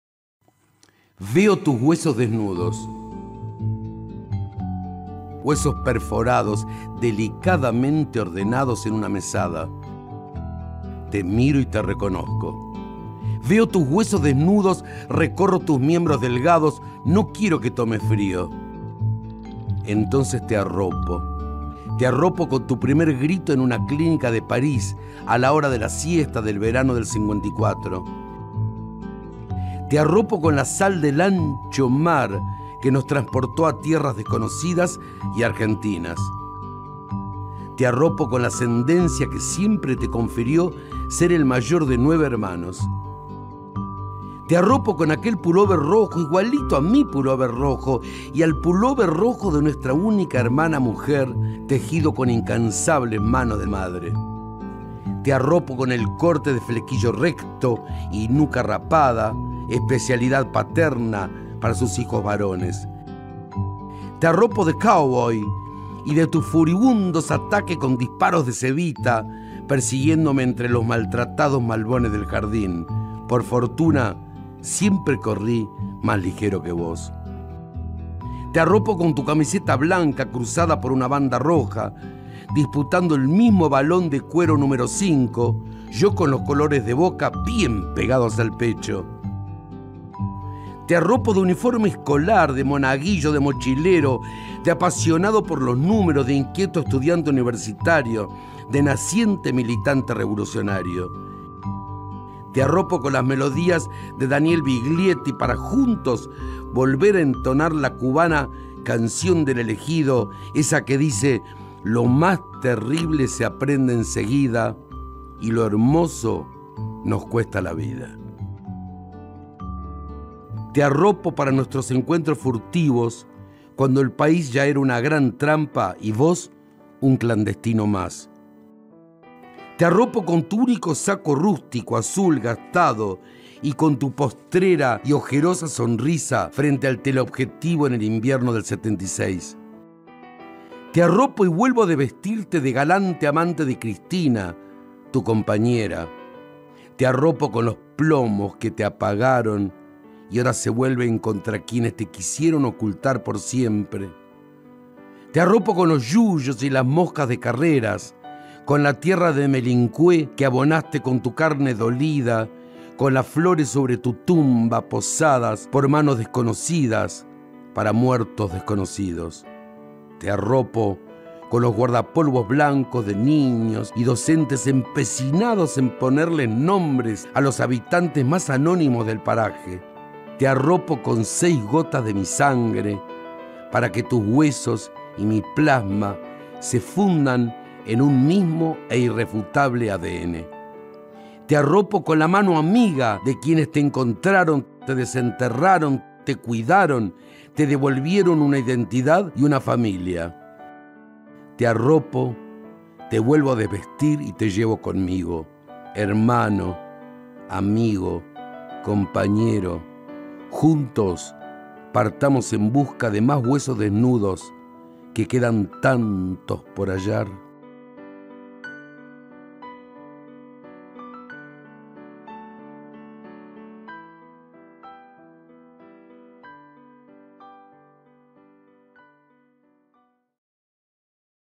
Huesos Desnudos - Eric Domergue
Lectura por Tom Lupo del poema "Huesos desnudos" de Eric Domergue. Trata de la desaparición en dictadura en Argentina y la identificación de los restos de Yves Domergue y Cristina Cialceta. Secuestrados y asesinados en septiembre de 1976 en Rosario, sus restos fueron identificados por el Equipo Argentino de Antropología Forense (EAAF) el 5 de mayo de 2010.